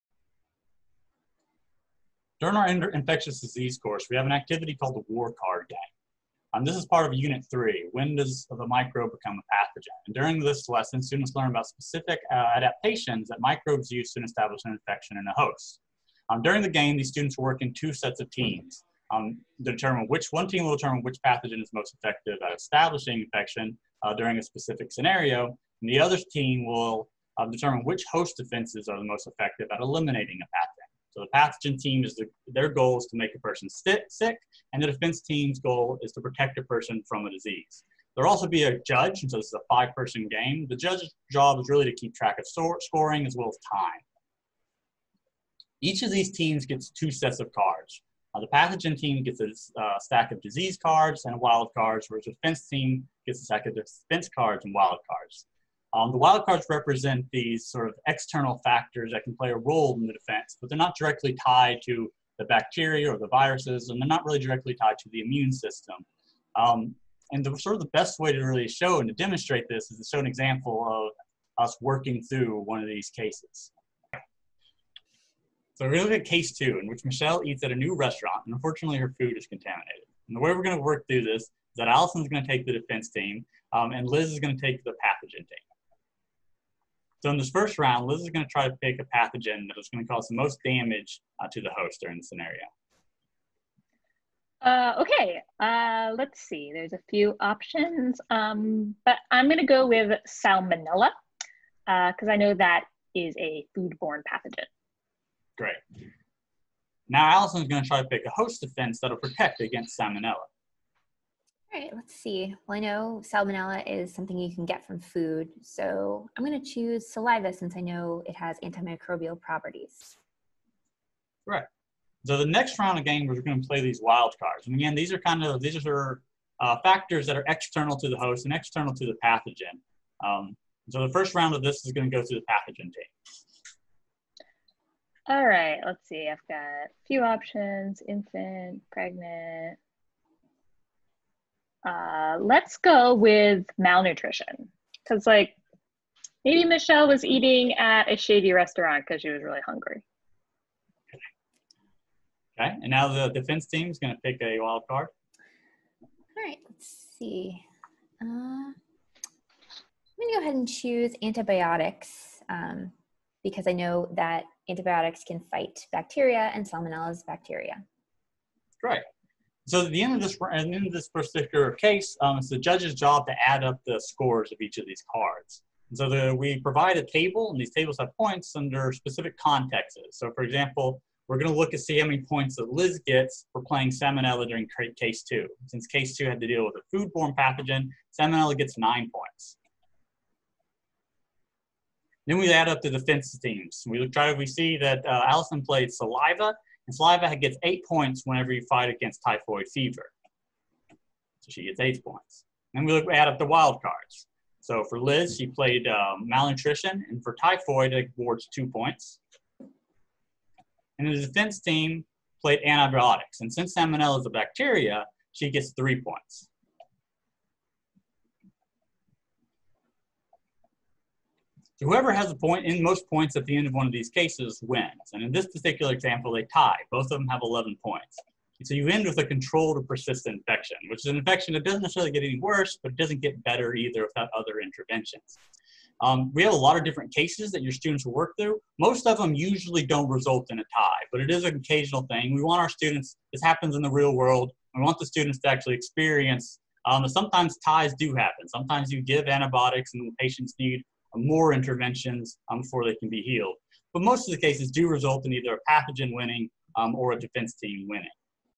Infectious Diseases High school activity - War Card Game
This video demonstrates a card game used in The Great Disease - Infectious Diseases, a free high school curriculum!

Produced by Tufts University School of Medicine Center for Science Education (CSE) as part of the Infectious Disease high school curriculum:

The Great Diseases curriculum provides a framework for students to learn high school biology in a more approachable and life-relevant context, designed to improve science engagement and health literacy.